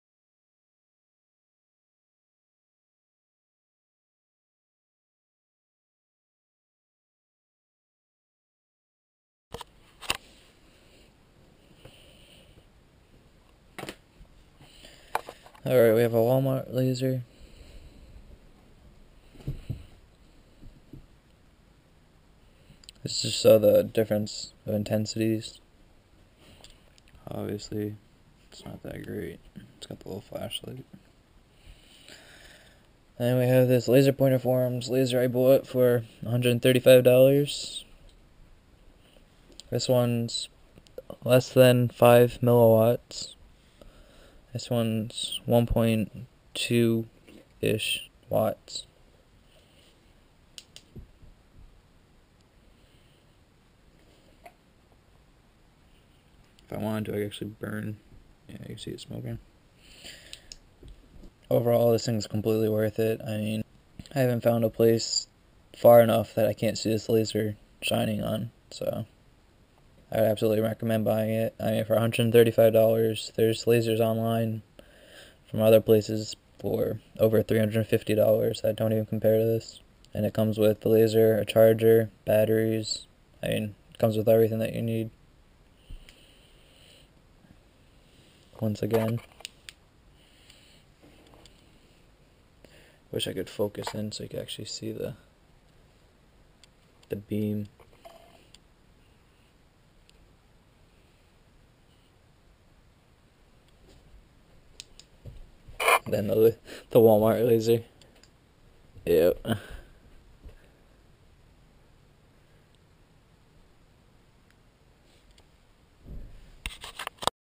laserpointerforums laser pointer
to get the laser from this video:

1: go to  LaserPointerForums.com
2: search for yobresal under members
3: send him a pm about his 1.2W copper 445nm laser pointers  (they say 1.2w but they are really about 1.3W)